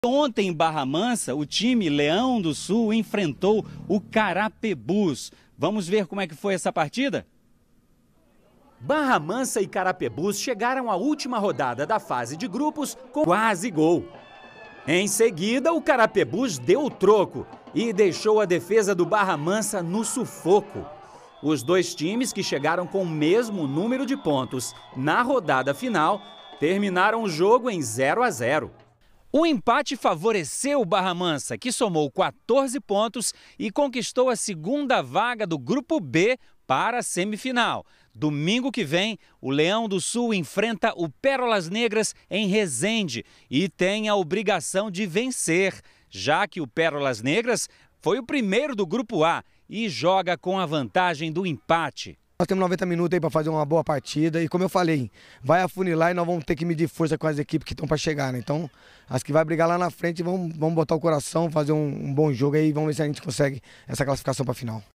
Barra Mansa: empate garante vaga na semifinal da série B2 do Carioca
Leão do Sul entrou em campo com cinco titulares substituídos depois da última partida que perdeu por 5 a dois pro time do Maricá.